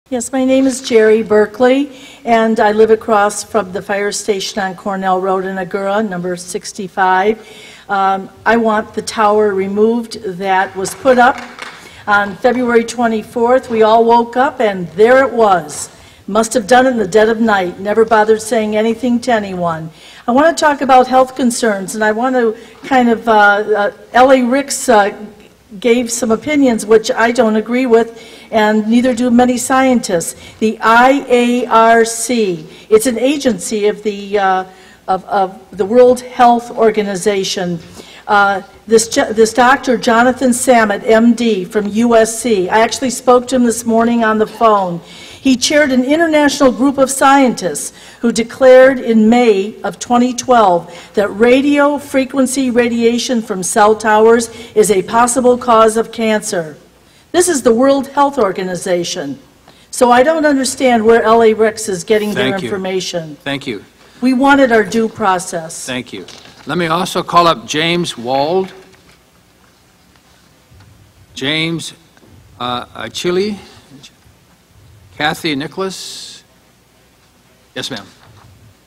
The health concerns of cell towers near homes
Citizen testimony opposing a cell tower a top a firestation near her home.